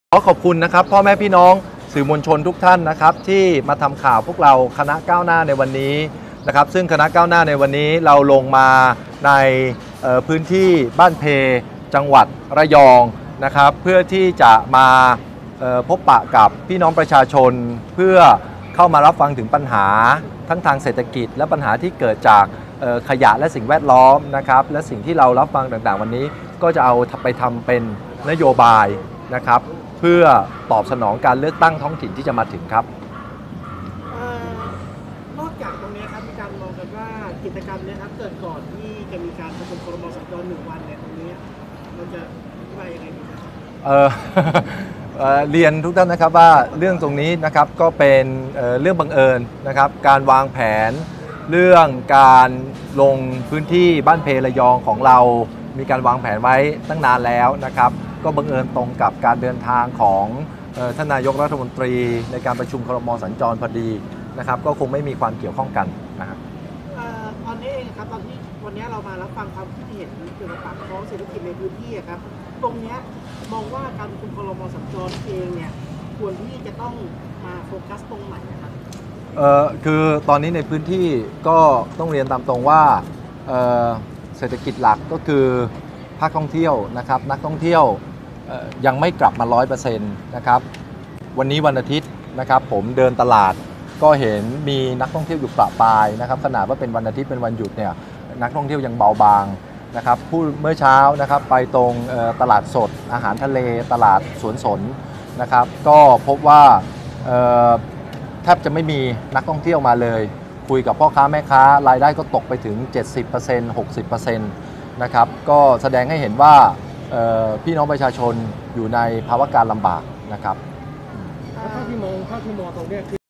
"ธนาธร"แจงลงพื้นที่ระยองวางแผนล่วงหน้ามานานแล้ว
นายธนาธร จึงรุ่งเรืองกิจ ประธานคณะก้าวหน้า แจงลงพื้นที่บ้านเพ จ.ระยองวางแผนล่วงหน้ามานานแล้ว ไม่เกี่ยวกับการประชุม ครม.สัญจรที่จะมีขึ้น